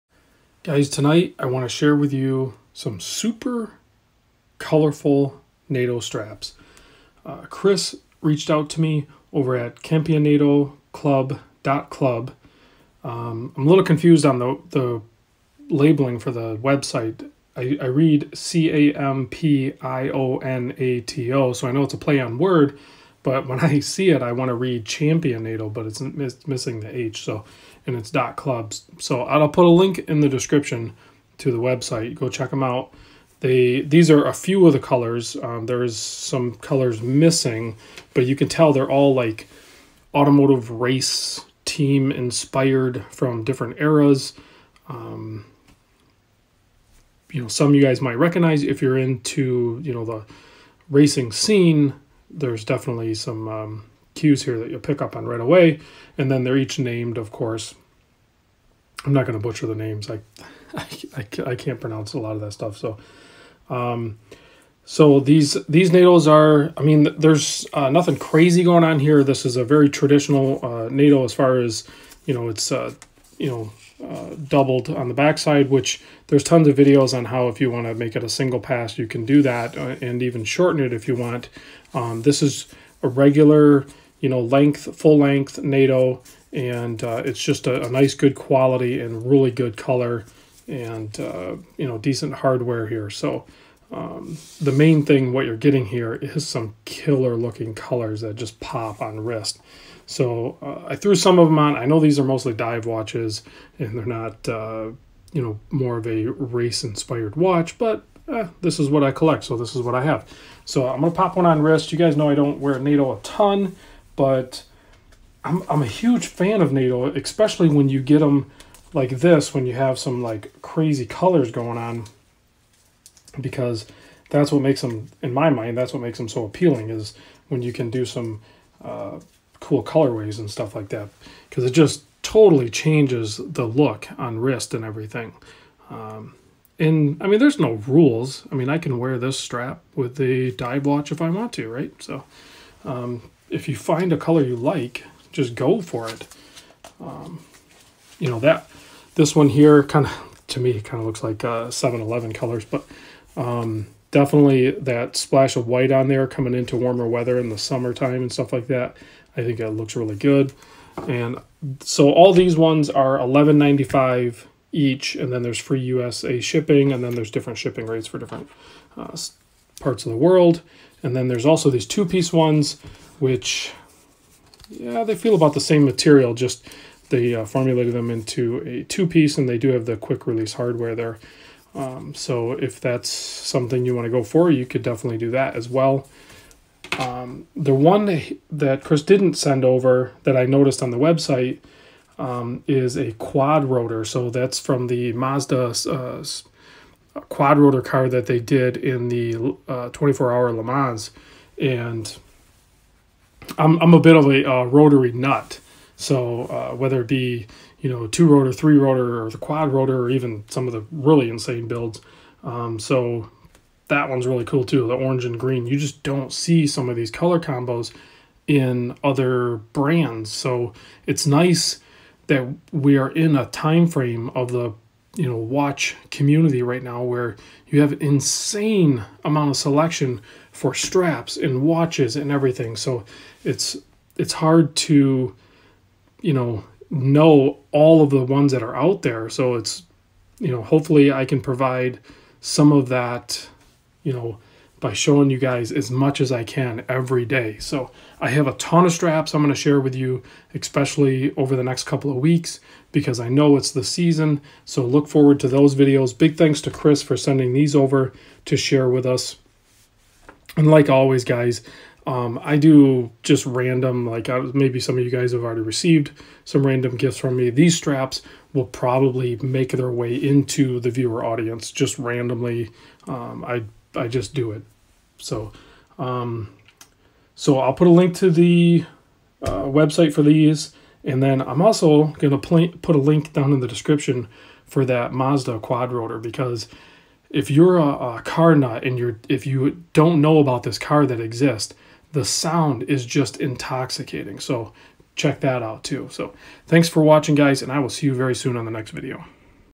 Race inspired Nato straps (Campinato.club)
Seriously check this video out, I absolutely love the sound of the Mazda 787b 4 rotor.